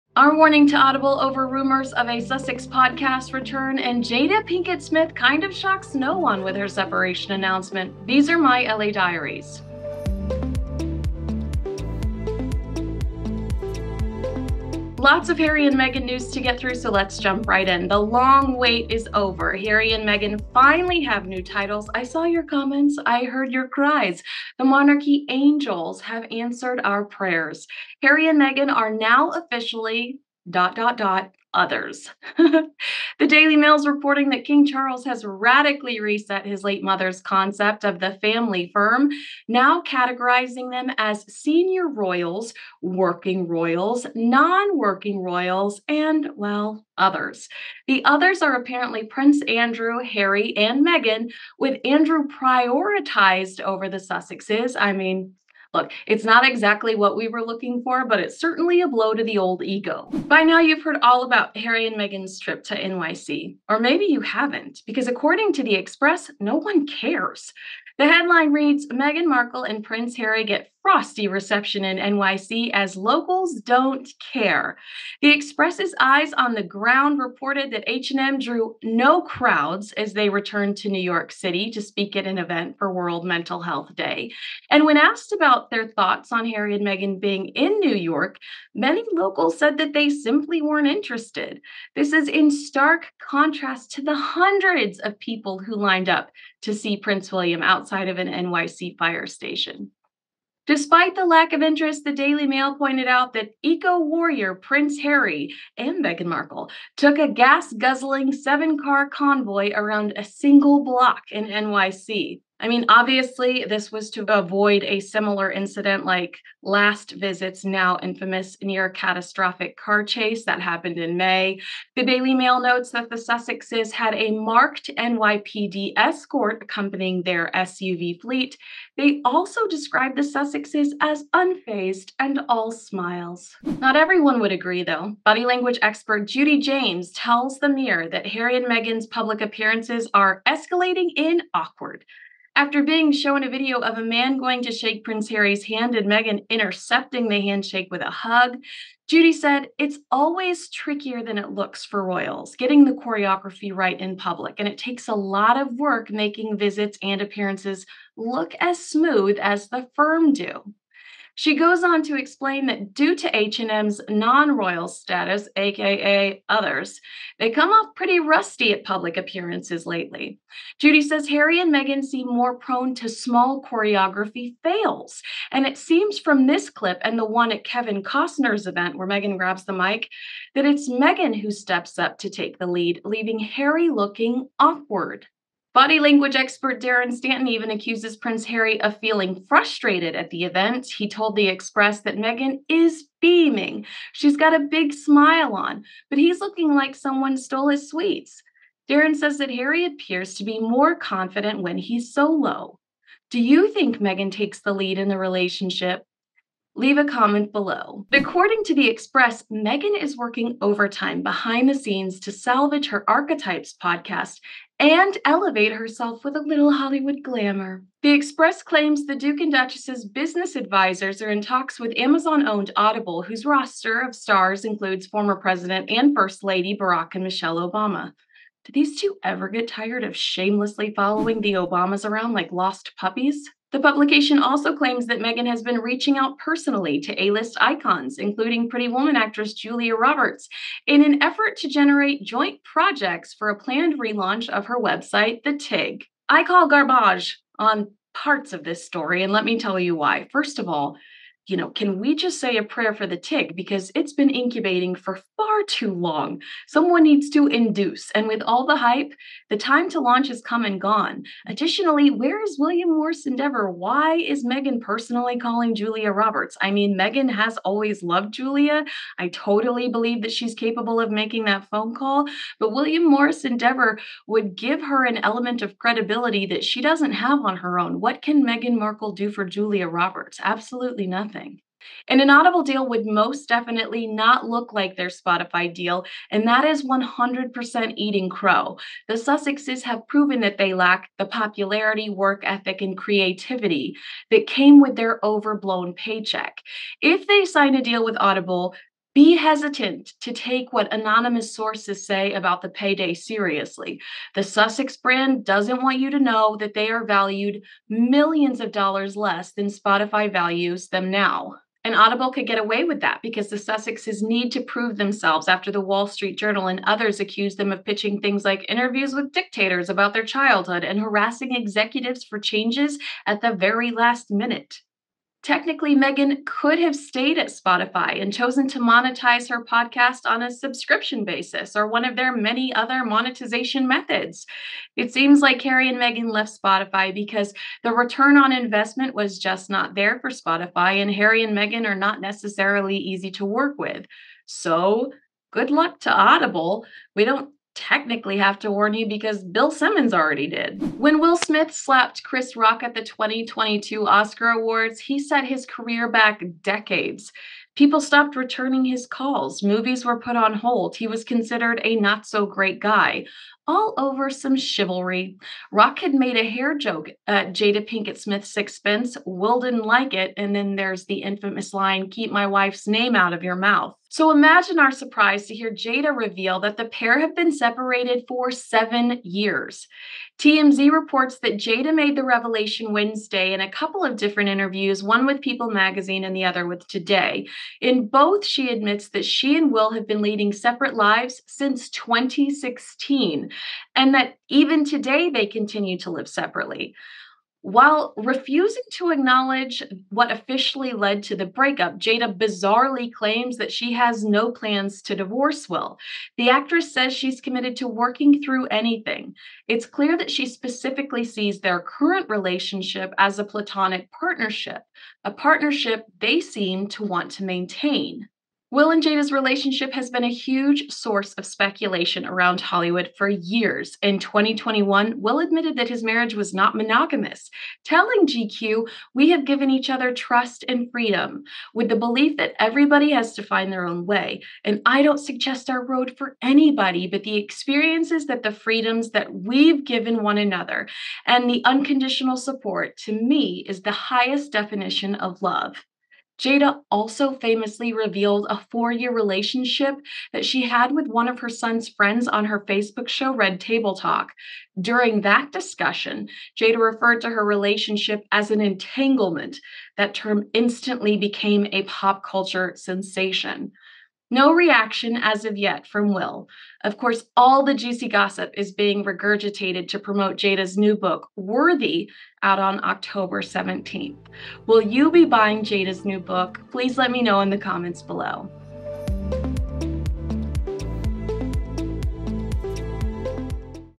Prince Harry And Meghan Markle's 'Failed' Body Language | Kinsey's LA Diaries
In the latest edition of Kinsey's LA Diaries, we delve into the intriguing world of Harry and Meghan, as they finally receive new titles and face a disappointing reception during their trip to New York City. Additionally, we explore Jada Pinkett Smith's shocking revelation about her separation from Will Smith. Let's dive into the juicy details.

After much anticipation, Harry and Meghan have finally been bestowed with new titles. The monarchy angels have answered our prayers, as they are now officially classified as "others," according to King Charles. However, this categorisation places Prince Andrew above the Sussexes, which is a blow to their ego. Though not exactly what they were hoping for, it marks a significant shift in the family firm's structure.

During their trip to New York City, Harry and Meghan received a frosty reception, according to reports. The Express claims that locals simply weren't interested in their presence, contrasting with the hundreds of people who lined up to see Prince William. Furthermore, the couple's use of a gas-guzzling seven-car convoy raised eyebrows, despite their eco-warrior image. Body language experts also noted a perceived awkwardness and choreography fails during their public appearances.